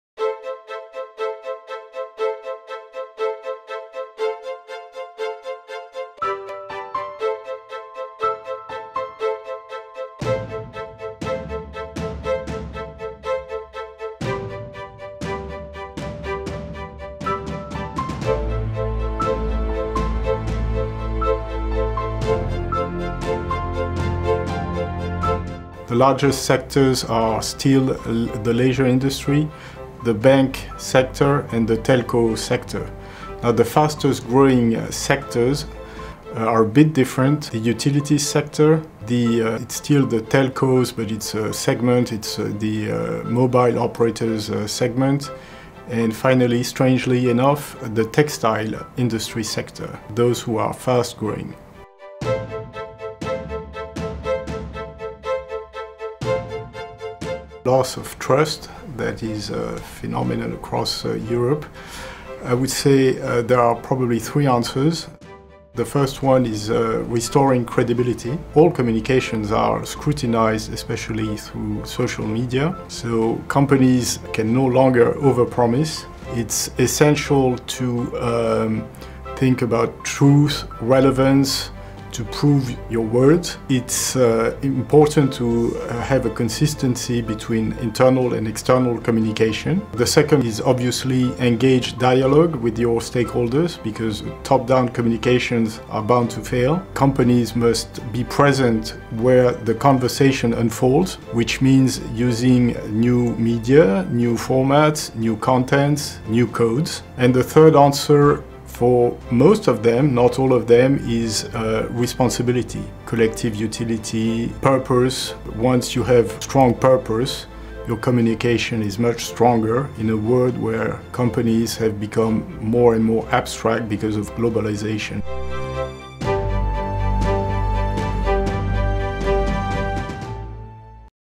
Fabrice Fries on Engaged Dialogue & Restoring Trust
MSLGROUP France President Fabrice talks about the opportunities in France, how credibility, dialogue and responsibility are the solutions to an economy suffering loss of confidence and how companies must use engaged dialogue with consumers.